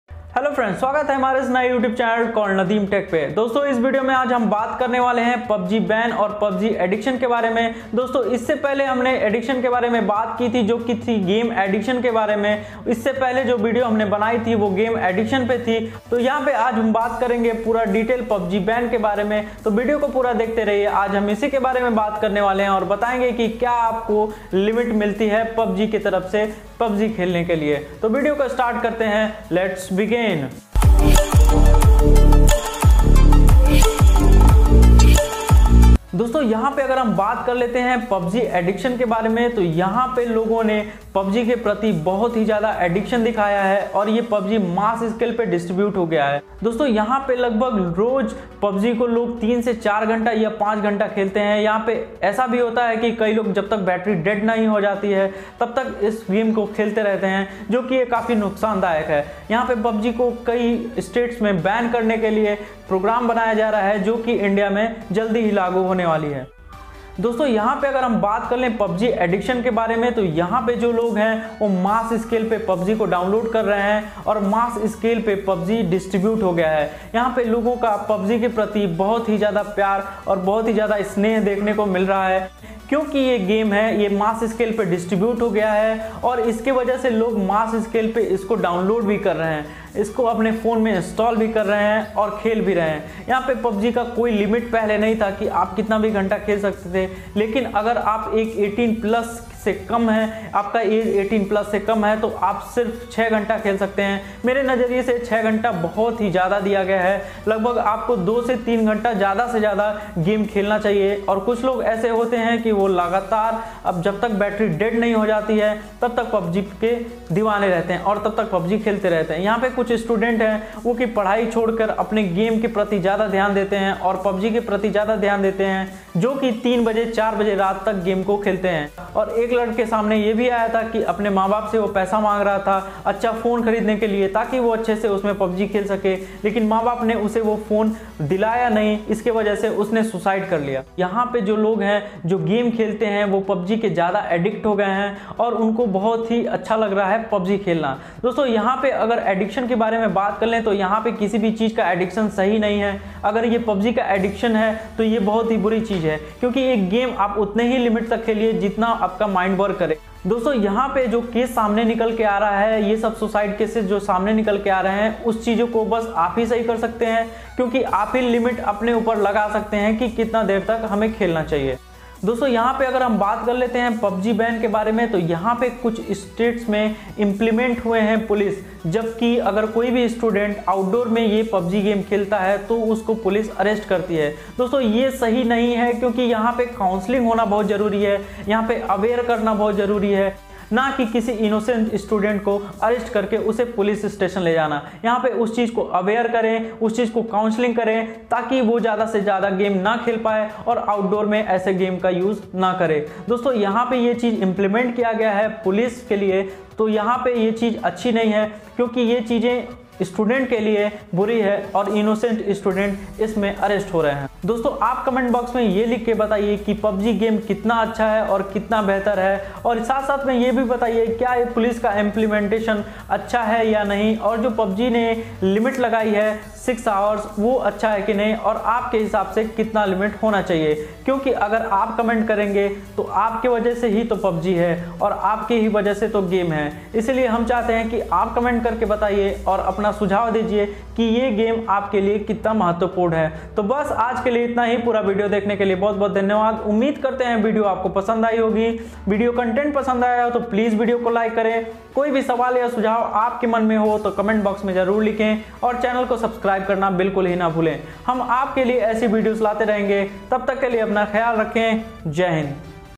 Pubg Ban !Pubg Suicide Case|| PUBG Arrest in India - PUBG 6Hr Playing My Opinion 🔥
Hello friends , is video mein maine aapse baat ki hai PUBG ke baare mein aur yaha pe bat kiya hai PUBG addiction ko aur saath hi saath PUBG arrest aur PUBG ki 6Hr ki limit ke baare mein, yaha pe maine aapse baat ki hai ki kaise aap isko sahi kr skte hai aur isme solution kya hai .Umeed krte hai ki aapko PUBG Mobile game ki yeh video pasand aayegi. JAI HIND

All the images which is shown in this video is taken from the is used only for the knowledge content not for the other purpose. all credit goes to google for the images